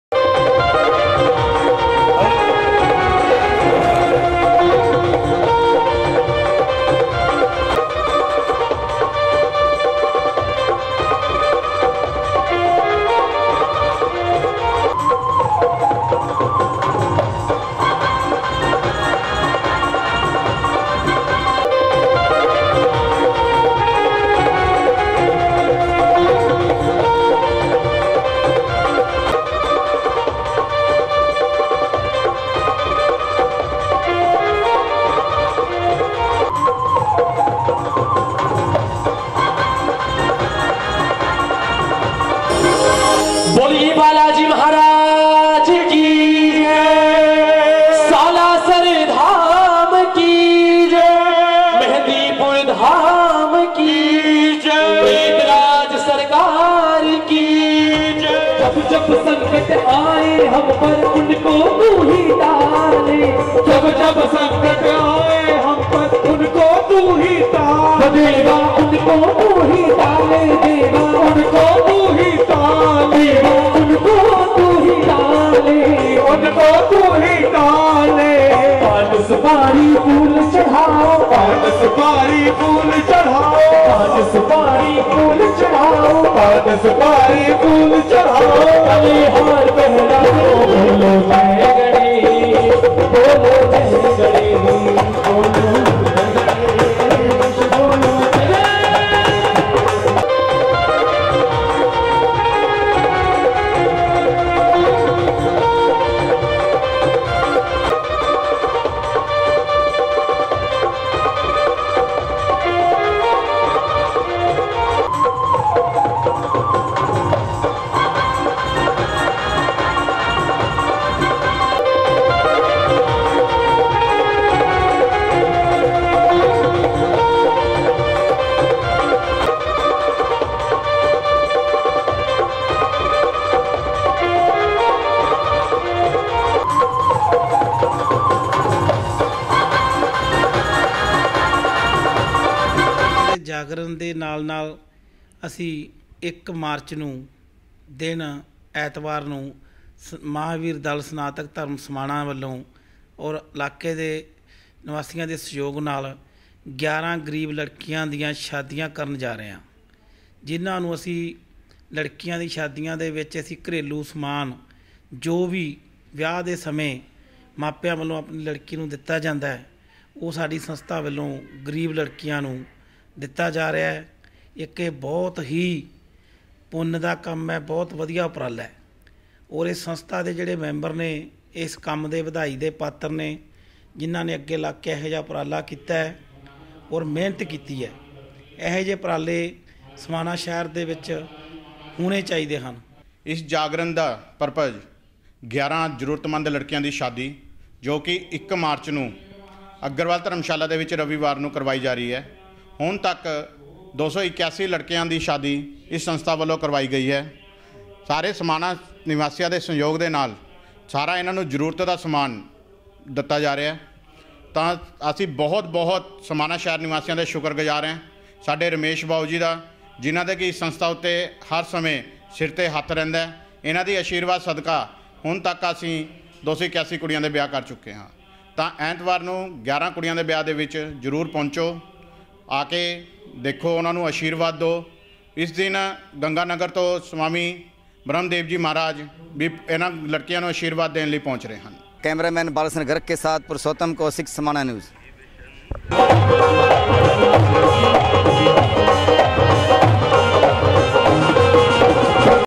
श्री सनातन धर्म महावीर दल ने करवाया सालासर बालाजी का जागरण 1 मार्च को 11 गरीब लड़कियों की शादी,..
समाना की अग्रवाल धर्मशाला में श्री सनातन धर्म महावीर दल पंजाब समाना ने सालासर बालाजी का जागरण करवाया जिसमें ज्योति प्रज्वलित श्री रमेश गर्ग इस संस्था के संरक्षक में की सर्वजीत सिंह बेदी ने पूजन करवाया इस मौके पर रजत गोयल एवं सर्वजीत बेदी ने बताया कि यह संस्था अब तक 281जरूरतमंद लड़कियों की शादी करवा चुकी है जिसमें कन्यादान के अंदर उसकी जरूरत का सभी समान दिया जाता है यह संस्था शहर वासियों के सहयोग से शहर के अंदर मां भगवती के जागरण एवं बालाजी महाराज के जागरण करवा कर उस राशि से गरीब कन्याओं की विवाह करवाती है

संस्था के सदस्यों ने शहरवासियों से अपील कि 1 मार्च को अग्रवाल धर्मशाला में पहुंचकर इन कन्याओं को अपना आशीर्वाद दें

beyt-1  सर्वजीत सिंह बेदी
beyt-2  रजत गोयल - श्री सनातन धर्म महावीर दल समाना  के सदस्य

पुरुषोत्तम कौशिक
Samana news